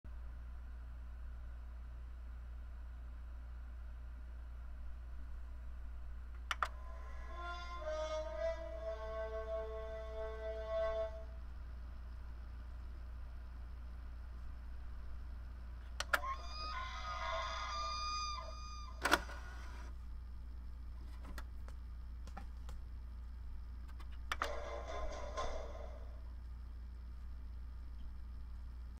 Gears of War 4 : XBOX One S : On/Off/Disc Drive Sound Effects!!!
Just thought i'd show how the sound effects work on the new Xbox One S Gears of War 4 Edition!!!